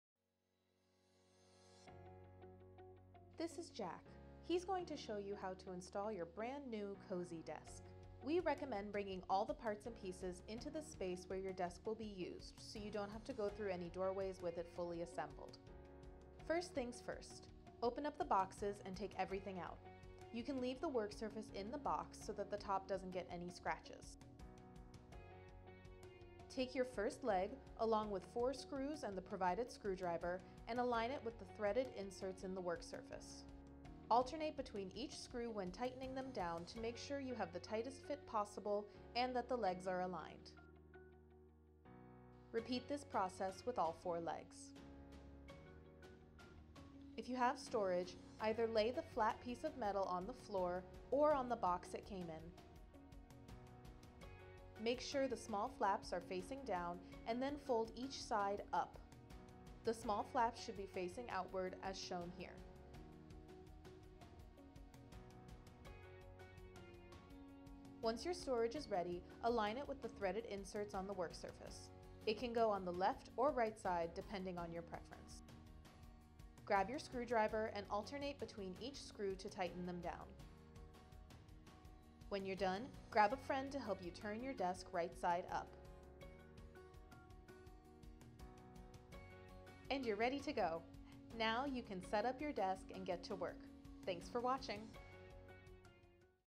Coze™ Desk | Installation Tips
Join Jack as he shows you how to put together a HON Coze™ desk.